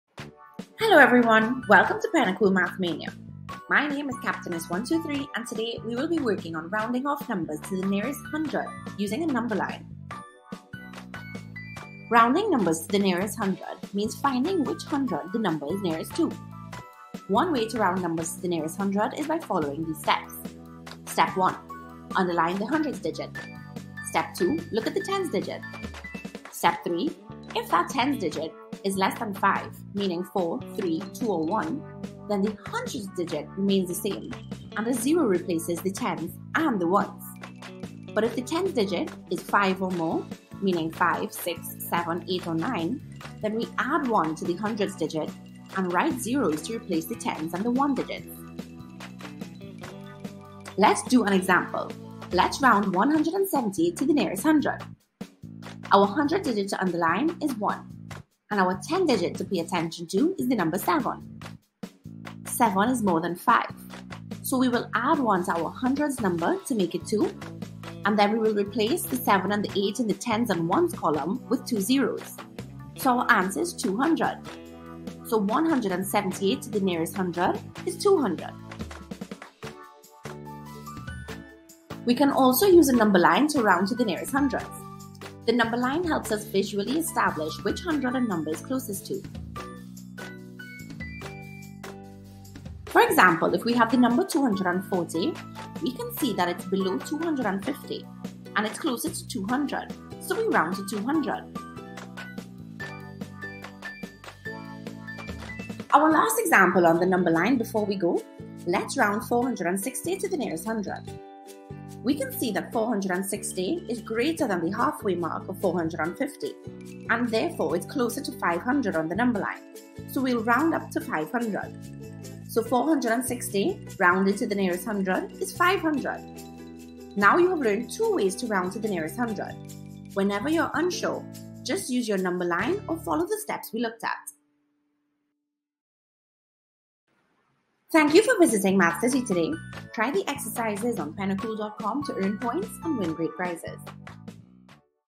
Rounding off to the nearest 100 (Standard 2) - pennacool.com
Join Captainess 123 as she helps round to the nearest 100 using a number-line.

Numbers - Standard 2

Log on to pennacool.com 123 Math & Language Arts Platform to access:
- FREE
- Self-correcting
- Curriculum aligned exercises
- Immediate Feedback